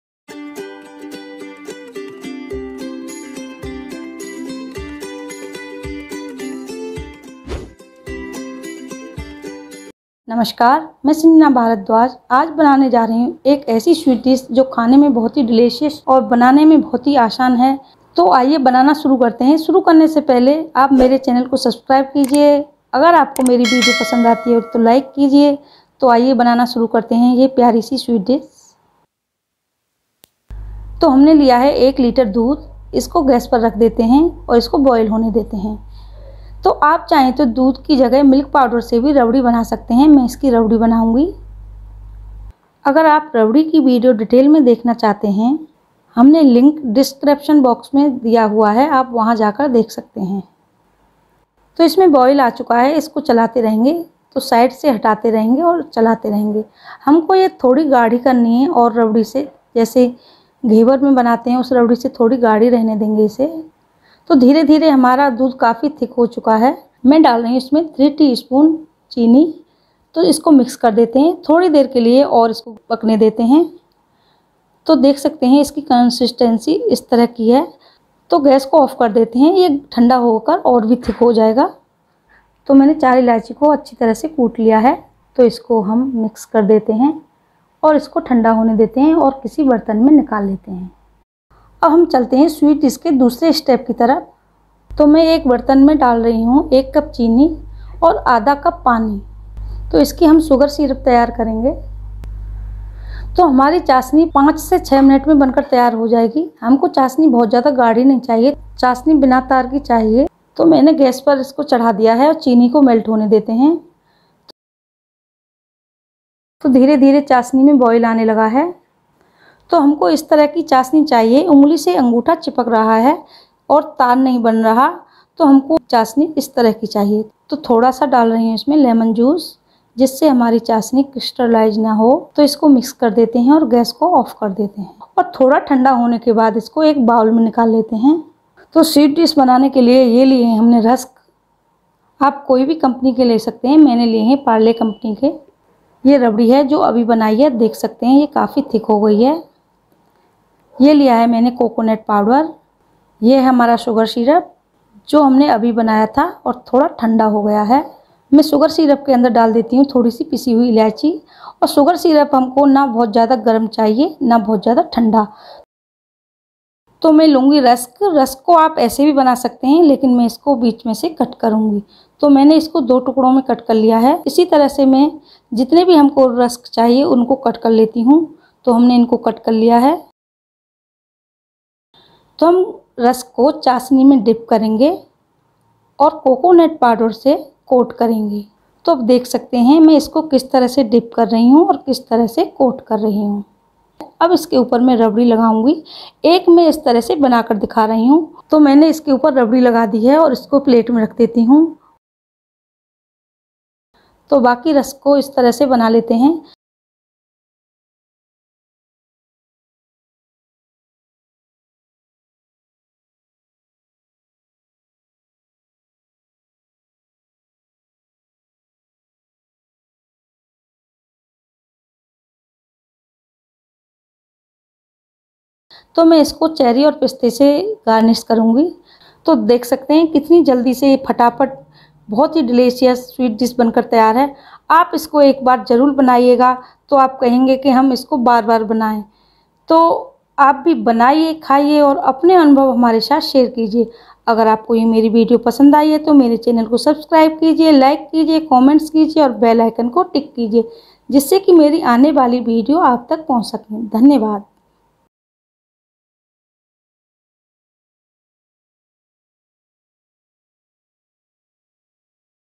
सिर्फ 10 मिनट में 4 चीज़ों से बनाये ये लाजवाब मिठाई | स्वाद ऐसा की भूल न पाए | Sanjana Bhardwaj
Hello friends
Today I'm making  very Special sweet Recipe. This is very easy and tasty Recipe ...
If you Follow this video Step by Step then I'm definitely sure that your sweet will made very Tasty..